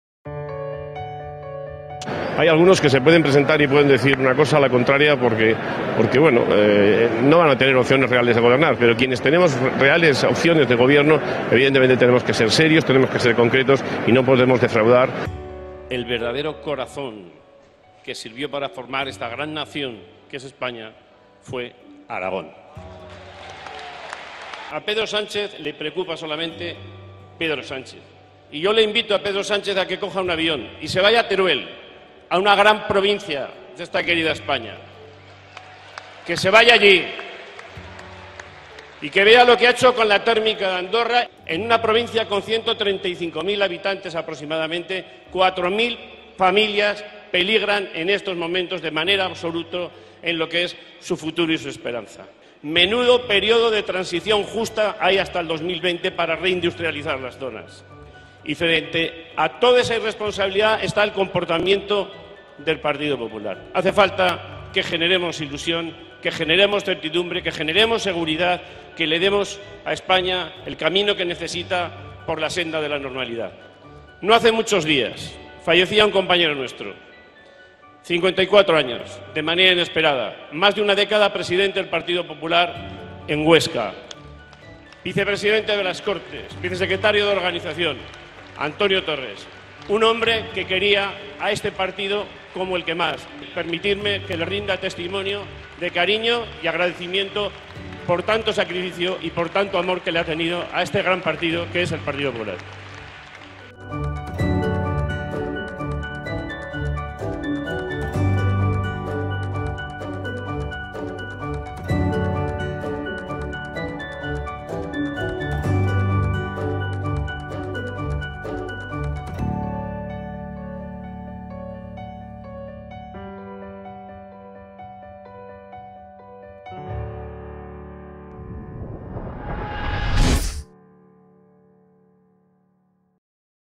Mejores momentos de EspañaEnLibertad 💙🇪🇸
📹▶️ ¿Quieres volver a ver lo ➕ destacado de la Convención del Partido Popular EspañaEnLibertad? 💙🇪🇸 Aquí tienes algunos de los mejores momentos de la delegación aragonesa de 'populares', con su presidente Luis María Beamonte al frente 👉🏻